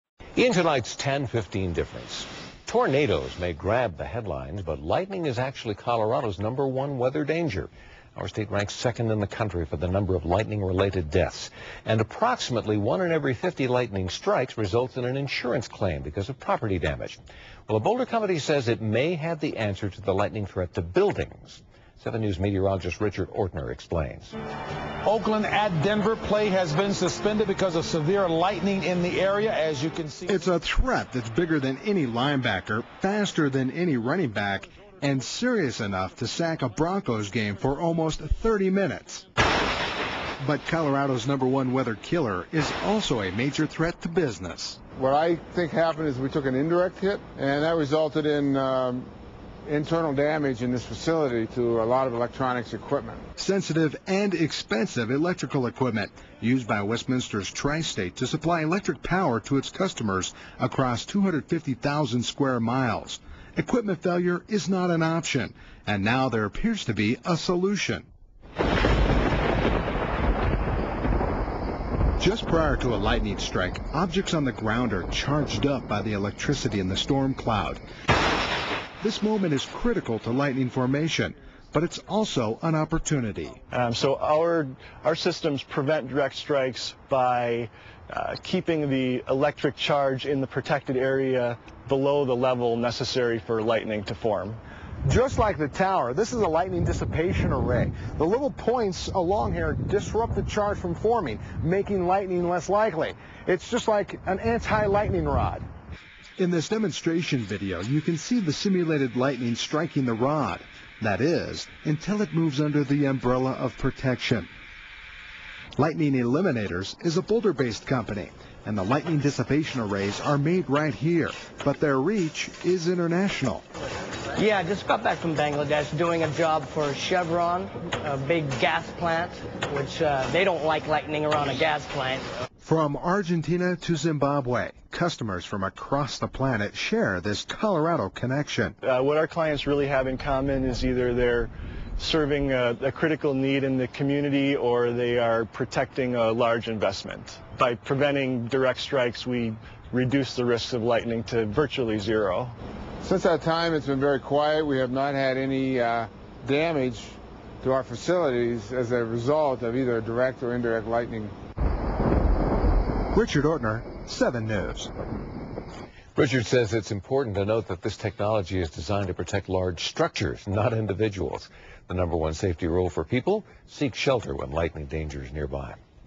Lightning Protection
Dan Hartt installing lightning protection equipment for over 30 years. Technology in lightning protection.